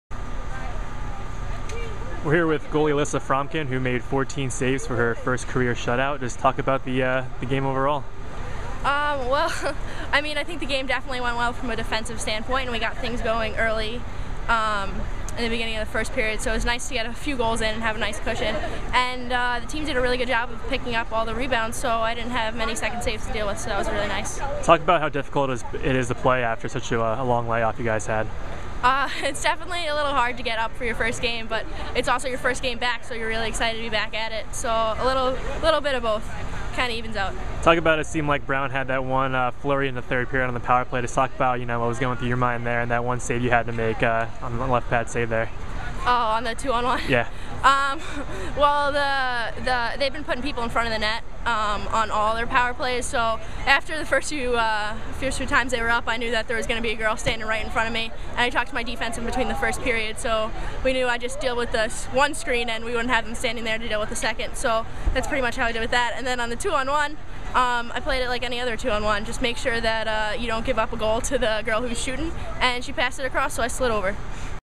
BU Women's Ice Hockey / Brown Postgame Comments
Sophomore Alissa Fromkin of the Boston University women's ice hockey team talks about the team's 4-0 win over Brown.